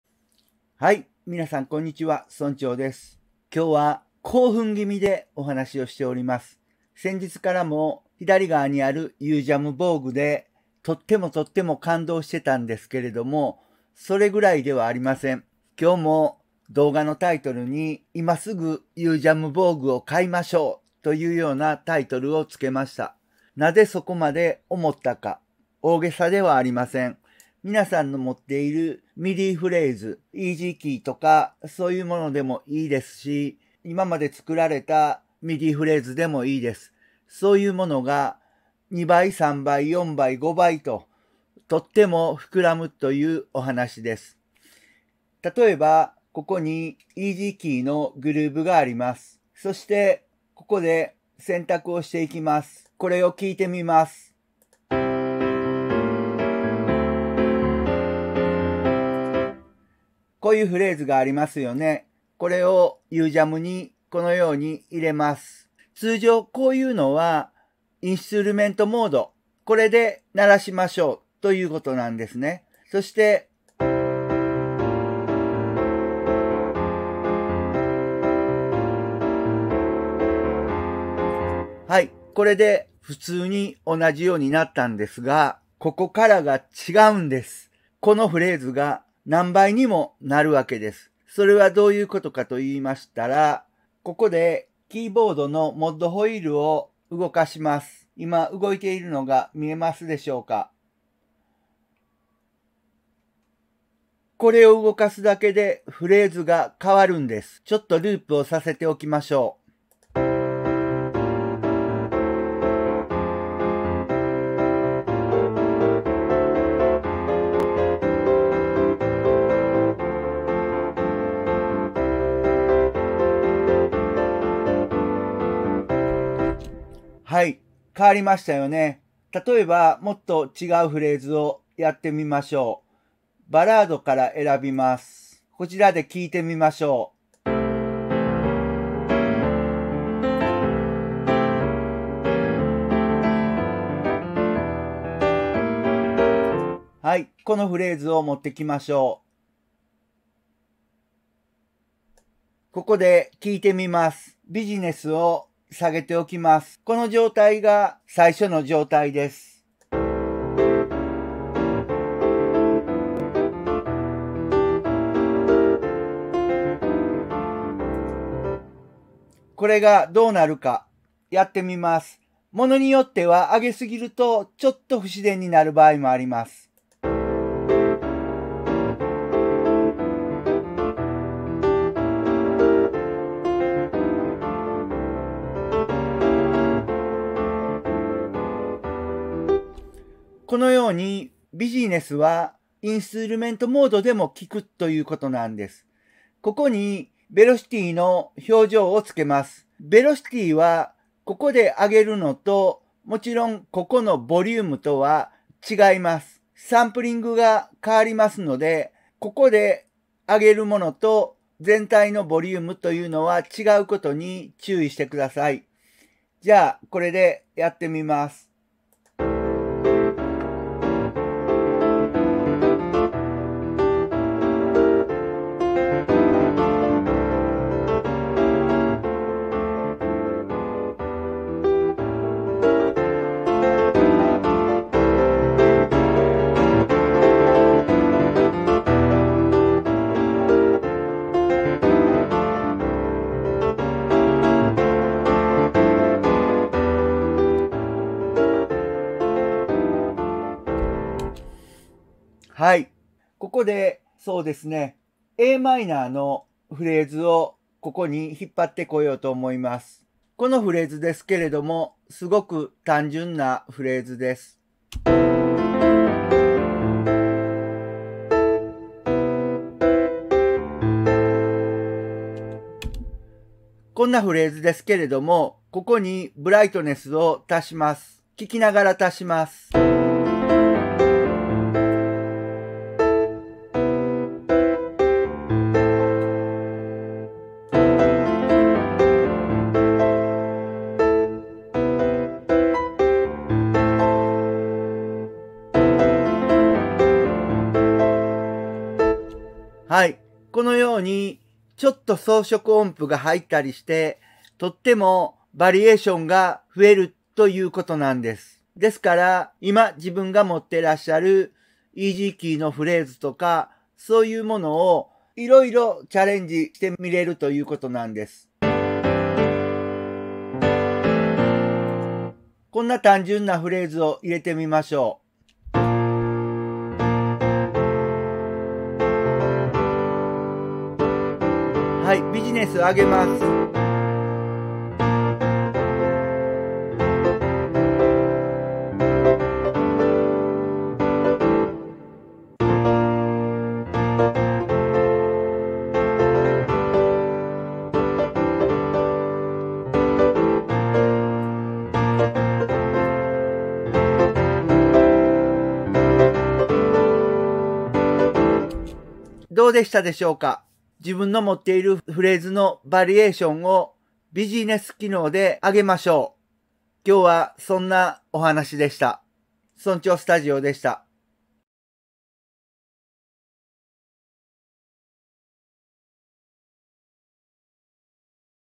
【English subtitles】VOGUE（UJAM）凄すぎる！ どんなMIDIフレーズにも自動でバリエーション追加 Busyness機能が圧巻 迷ってるなら今すぐ買おう
絶対見るべし！！
VOGUEのBusyness機能
迷ってるならVOGUE買おう